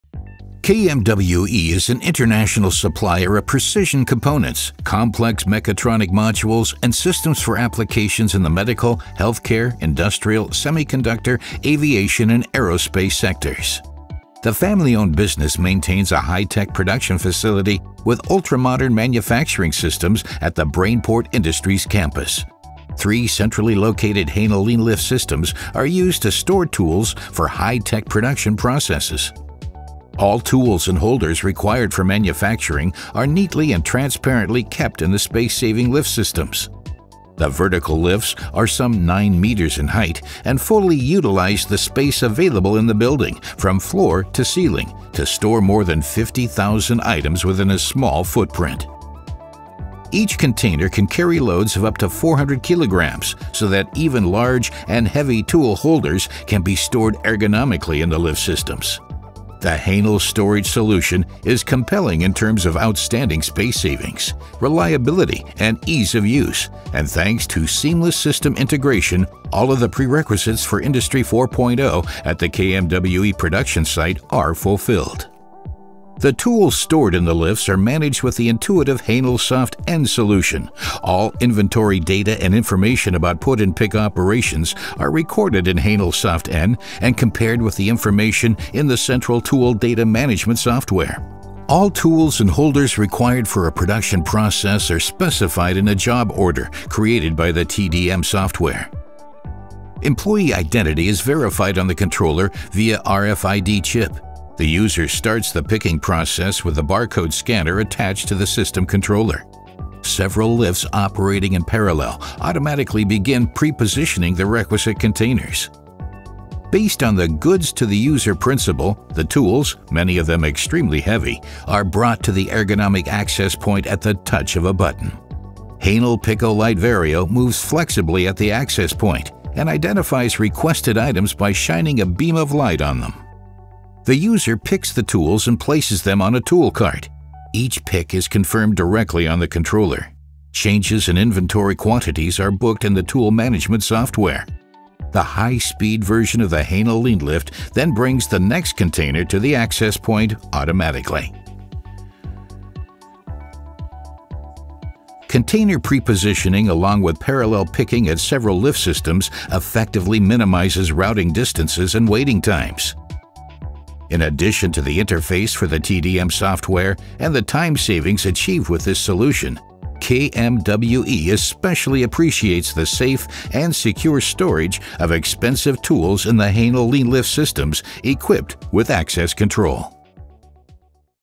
The Hänel Lean-Lift® as highly efﬁcient tool depot in high-tech production
Three high-speed Hänel Lean-Lift® systems serve as the central 24/7 tool depot for 60 machining centers in the production area